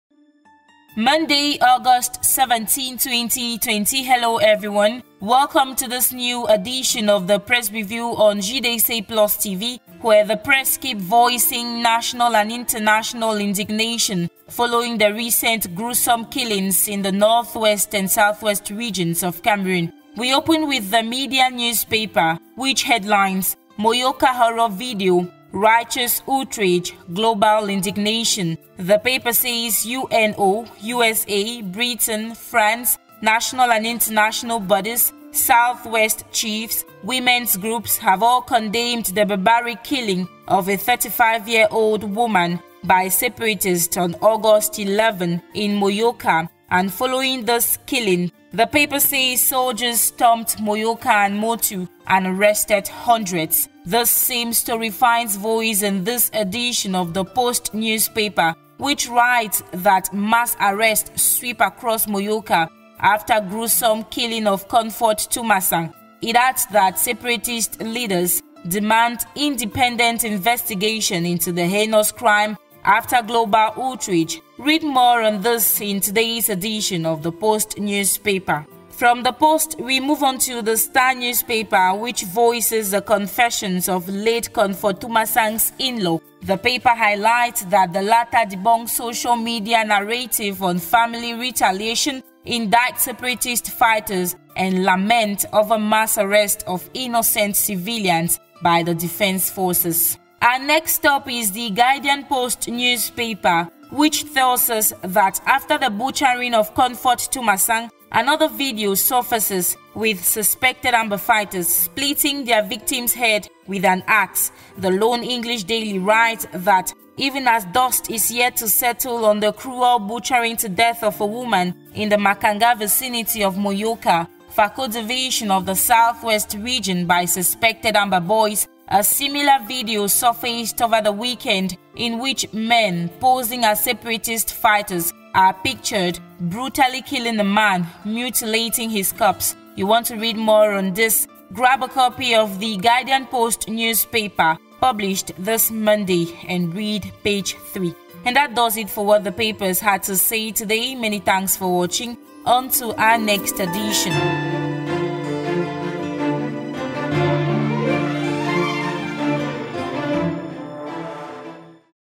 CAMEROONIAN PRESS REVIEW OF AUGUST 17, 2020
The barbaring killing of a 35-year-old mother of four, by separatists in Muyuka continues to shock and distress Cameroonian tabloids. Get to know more in our press review.